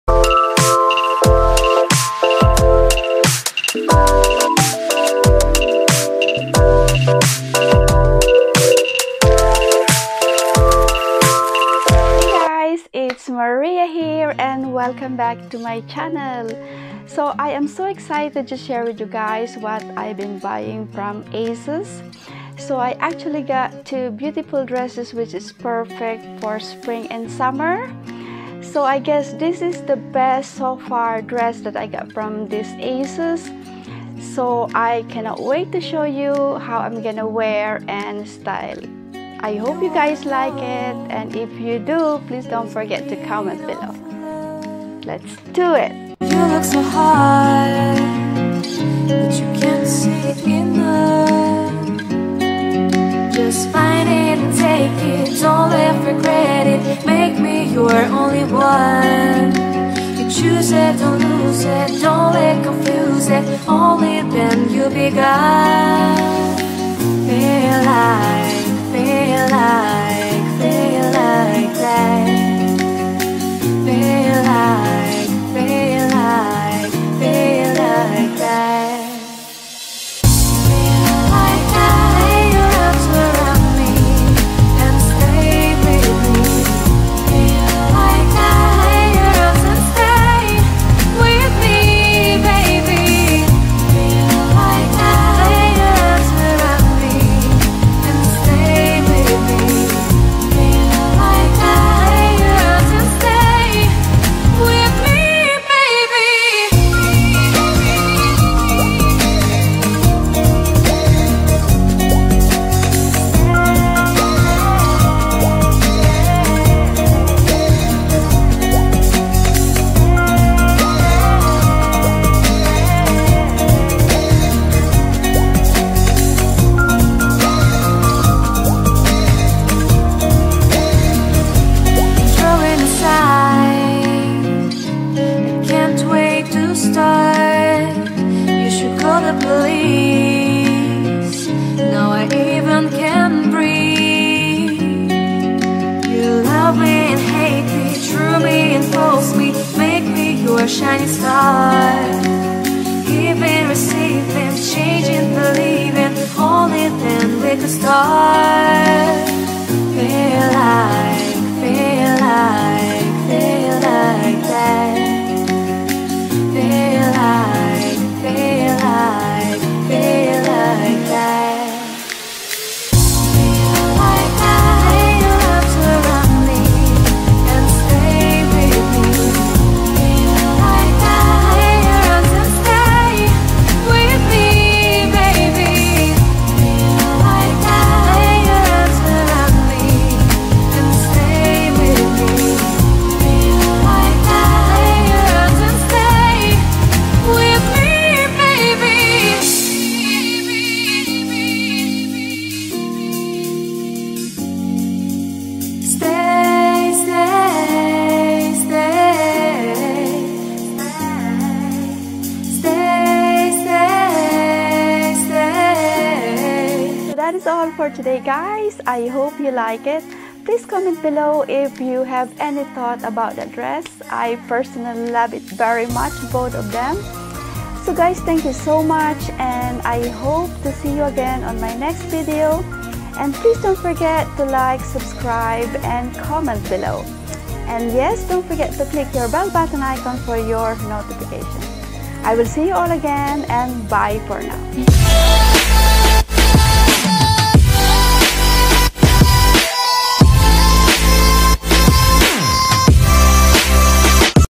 Beautiful Asos dresses it's on SALE
Its a gorgeous sunny day here in ireland and its just perfect that I got this dresses from Asos which is a mazingly beautiful in anyway. The quality are perfect, please watch the video till the end and let me know if you have any thoughts on that two beautiful dresses.

. White broderie tea maxi dress with puff sleeve

. Broderie knot front maxi dress in mustard